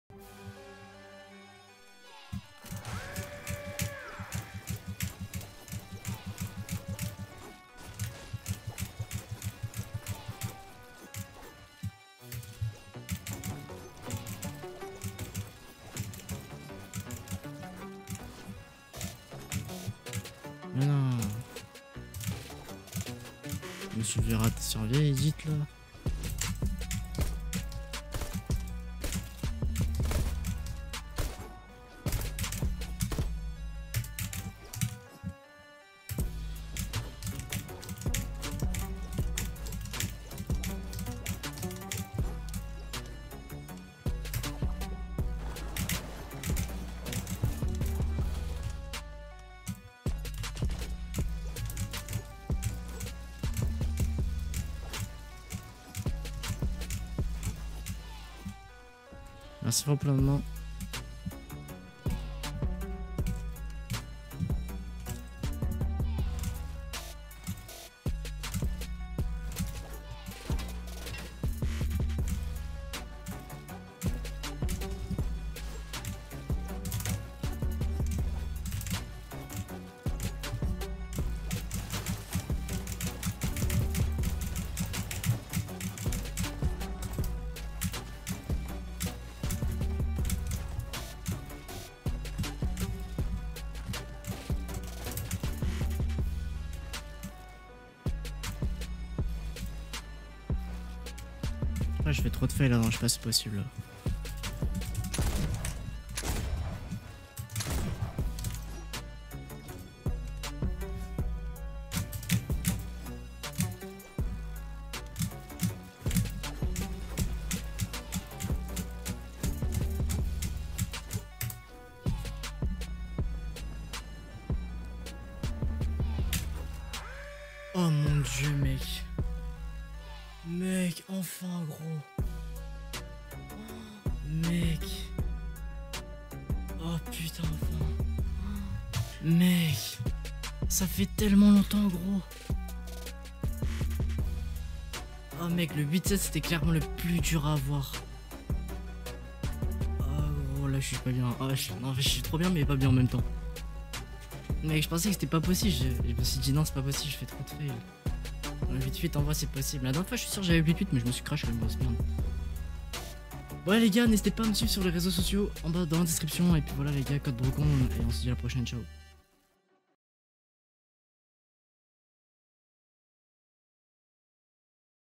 8:07!! WORLD RECORD BouziTournament3 | HANDCAM | ALMOST PERFECT RUN !
Yo les potes !✌️ Enfin le 8:07 sur le BouziTournament3 !!

👍N'hésite pas à mettre le code créateur BROCO pour me soutenir ! 💪
🔥Merci INFINIMENT à tous ceux qui le mettront ! ❤️